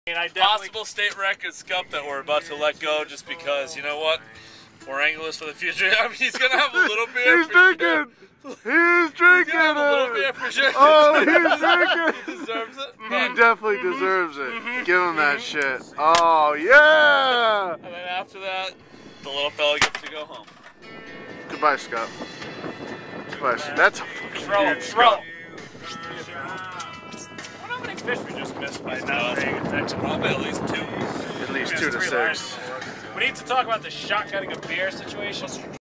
The Drunk Scup
This was a giant scup we caught off Point Judith, and boy was he thirsty.